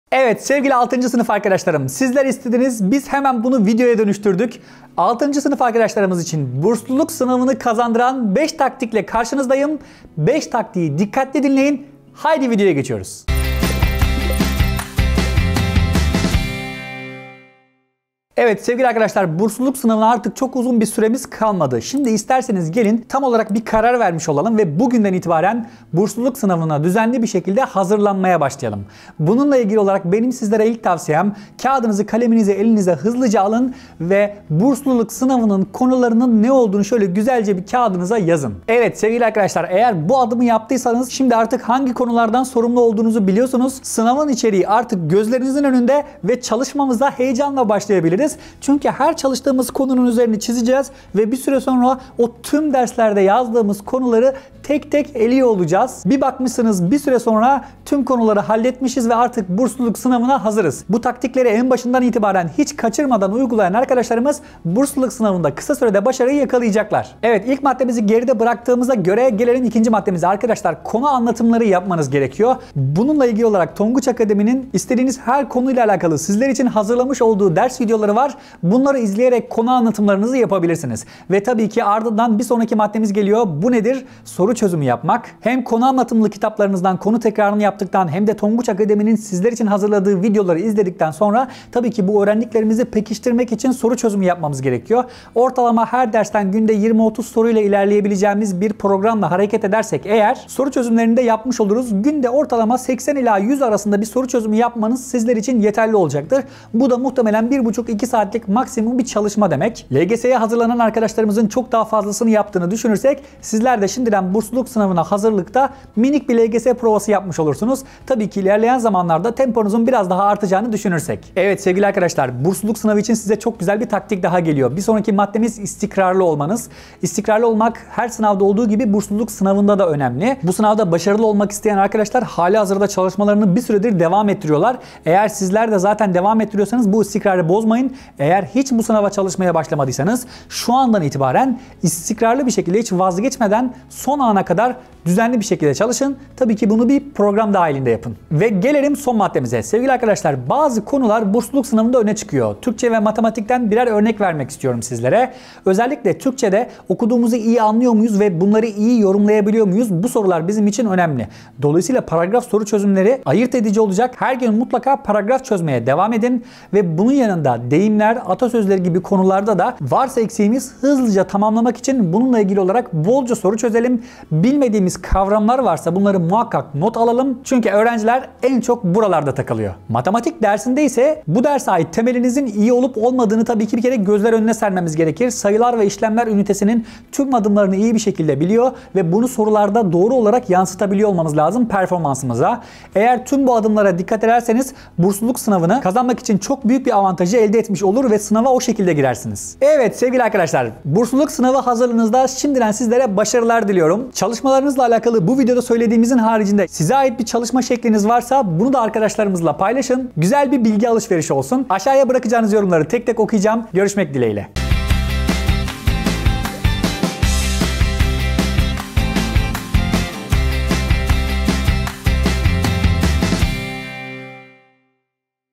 Bursluluk Sınavını Kazandıran 5 Taktik 🎯
Bursluluk sınavına hazırlanırken nelere dikkat etmelisin? Sınavı kazanmak için neler yapmalısın? Bursluluk sınavını kazandıran 5 taktiiği videomuzda anlattık. 🧐